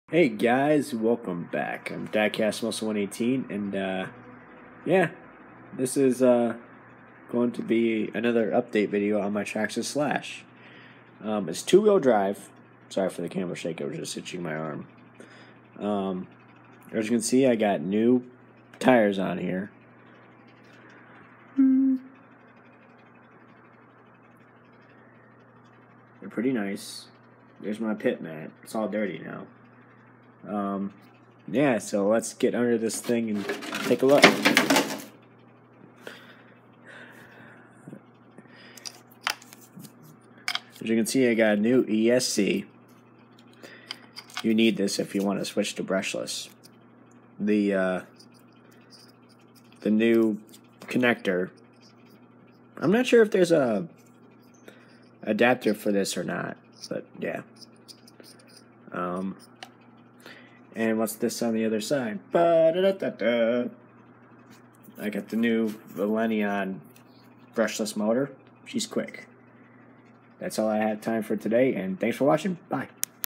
Traxxas slash update
The update is a brushless motor and esc